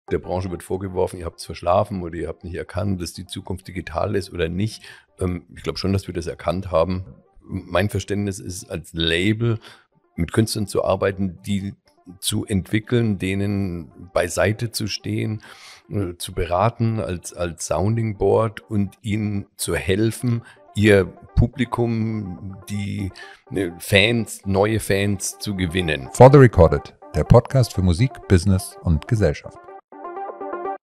For the record(ed.) - Ab sofort überall, wo es Podcasts gibt!
Der Podcast für alle Menschen, die sich für die Musikbranche begeistern, und für all diejenigen, die mehr über jenes Business erfahren wollen, das ihre Lieblingslieder und -alben hervorbringt: For the record(ed.) - Der Podcast für Musik, Business & Gesellschaft 🎧 🎶